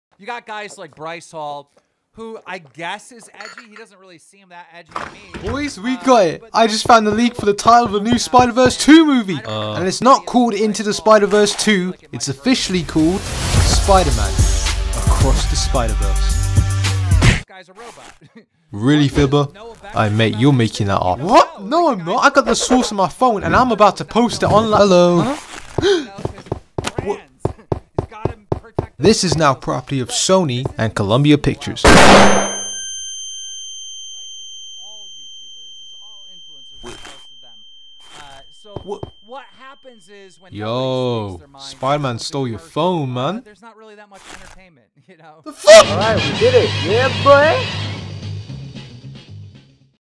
Spider-Verse 2 *OFFICIAL Title Leaked*
In this LEGO Video, there is a leak for the Spider-Verse 2 movie that gets leaked out!

There was leak by ComicBookCast2 finding info that the sequel to "Spider-Man: Into The Spider-Verse" is called "Spider-Man: Across the Spider-Verse". I decided to make a LEGO Short Skit about it because I had nothing else to do lol.

A young adult called Alex walks into his house w/ his friends saying that he has the Spider-Verse 2 Title and he's about to post it online when out of nowhere, LEGO Spider-Man himself appears with smaller eyes and a fancy cape for some reason?
He takes the phone (which has the "Into the Spider-Verse" Title on it" and teleports out of the house leaving Alex feeling shocked and upset that the info got taken from him.

This isn't a LEGO Spider-Man x Miles Morales video but it is a LEGO Spider-Verse video which isn't related to that series. I will get that LEGO Spider-Man x Iron Man video that I'm doing but I'm waitng for lines from a special guest at the moment! But in terms of this Spider-Verse 2 video, this is probably just the one off video / experiement video that I would likely make every now and then.

Music Used

Links